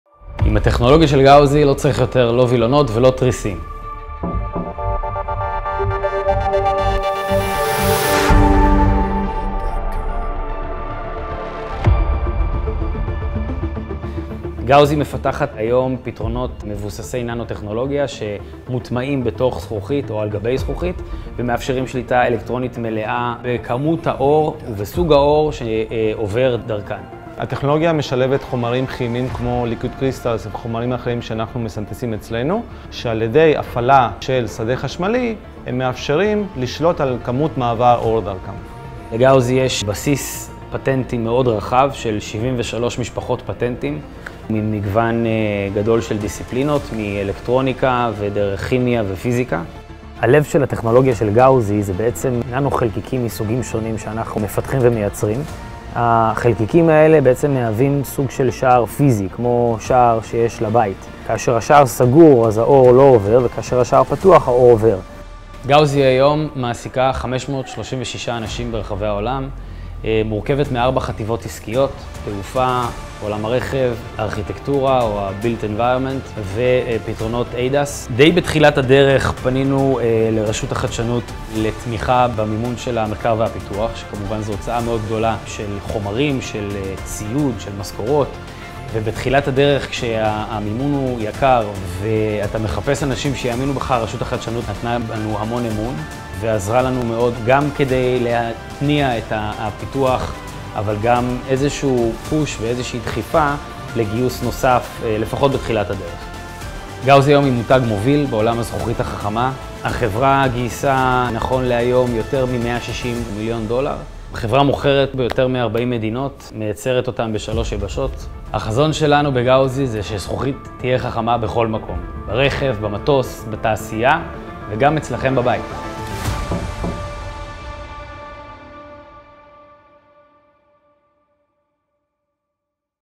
עם הטכנולוגיה של Gauzy - כבר לא צריך וילונות או תריסים!
הכירו את Gazuy – חברת ננו טכנולוגיה ליצירת זכוכיות חכמות, מהי זכוכית חכמה? Gauzy מפתחת פתרונות חכמים, ננו טכנולוגיים, המוטמעים בתוך זכוכית או על גביה ומאפשרים שליטה מלאה על כמות וסוג האור העוברים דרכה באמצעות הפעלה של שדה חשמלי.
הלב של הטכנולוגיה של Gauzy הוא בננו חלקיקים, המפותחים ומיוצרים על ידם במעבדה ורשומים ברשימה ארוכה של פטנטים. החלום של Gauzy, יתגשם כאשר נמצא את הזכוכית שלהם בכל מקום – ברכב, במטוס, בתעשייה ואפילו אצלכם בבית!
ההשקעה מרשות החדשנות במחקר ופיתוח עוד בראשית הדרך של Gauzy נתנה לה את האמון והעזרה להתניע את הפיתוח. Gauzy היום הינה מותג מוביל בעולם הזכוכית החכמה, גייסה יותר מ-160 מיליון דולר, מעסיקה מעל 500 עובדים, מוכרת ביותר מ-40 מדינות ומייצרת בשלוש יבשות!